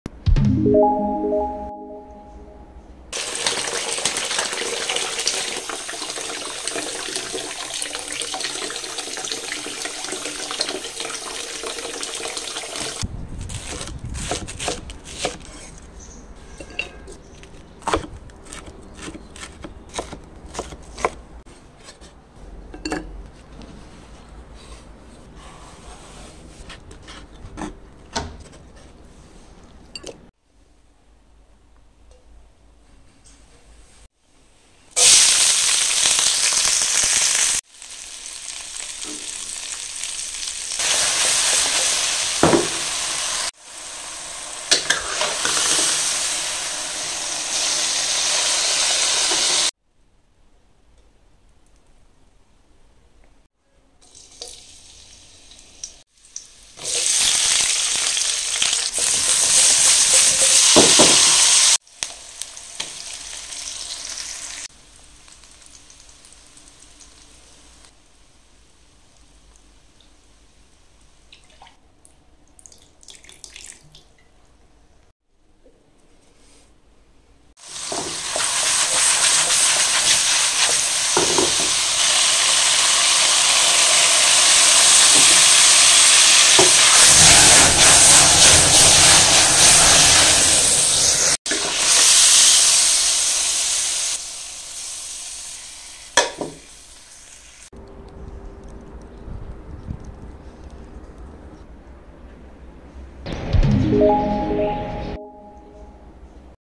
白菜青椒炒猪肝，青椒炒猪肝的汤汁渗入大白菜，大白菜的味道便变的鲜美脆嫩起来，青椒猪肝依然脆爽鲜甜。Stir-fried pork liver with cabbage and green pepper
白菜青椒炒猪肝的做法：
1，材料：猪肝，青椒，大白菜，
2，辅料：油，盐，姜丝，耗油，生抽，黄酒，白酒，胡椒粉，
3，青椒炒猪肝的汤汁渗入大白菜里，大白菜的味道便变的鲜美脆嫩起来，青椒，猪肝依然脆爽鲜甜。